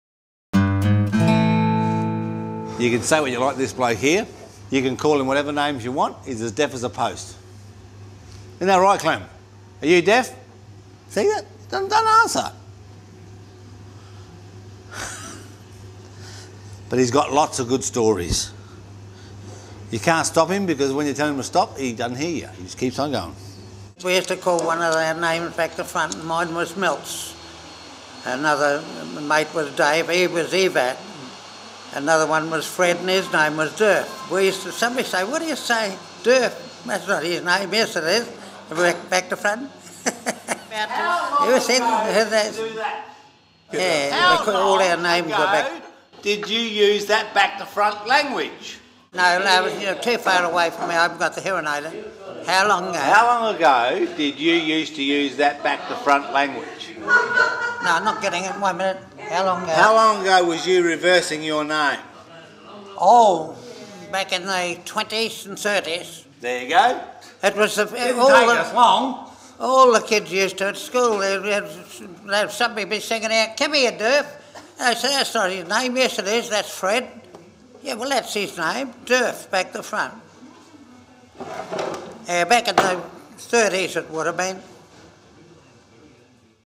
91 year old Clem in the Port Augusta Men's Shed: Big Stories, Small Towns
Clem might be stone deaf but he's got a lot of good stories to tell from way back in the '20s and '30s. And you have to listen, because he can't hear you if you ask him to stop. He's not bad with a set of tools. either.